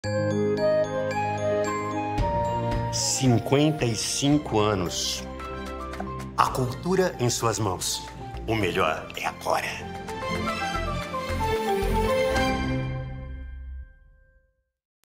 O MELHOR É AGORA | Cassio Scapin
O ator Cassio Scapin se une à TV Cultura em um projeto especial para celebrar os 55 anos da emissora.